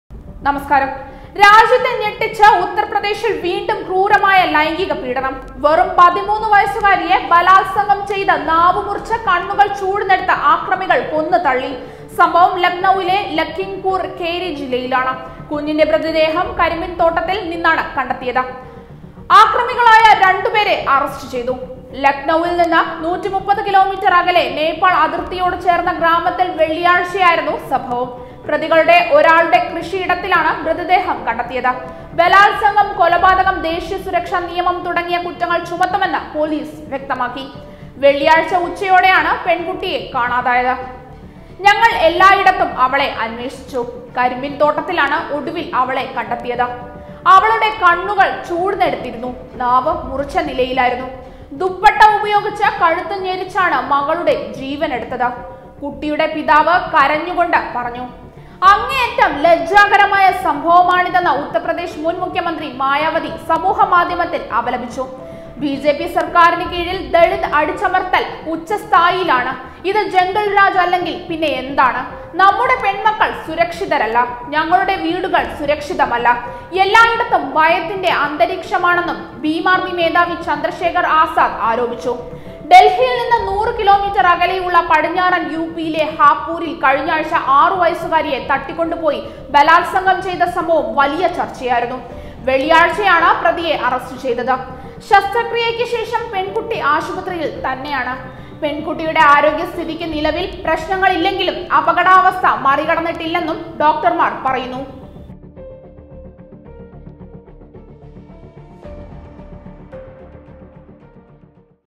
കണ്ണില്ലാത്ത ക്രൂരത- 13 വയസ്സുകാരിയെ ശാരീരികമായി ഉപദ്രവിച്ച് കൊന്ന് തള്ളി
കണ്ണില്ലാത്ത ക്രൂരത- 13 വയസ്സുകാരിയെ ശാരീരികമായി ഉപദ്രവിച്ച് കൊന്ന് തള്ളി, മൃതദേഹത്തിൽ കണ്ണും നാവുമില്ല !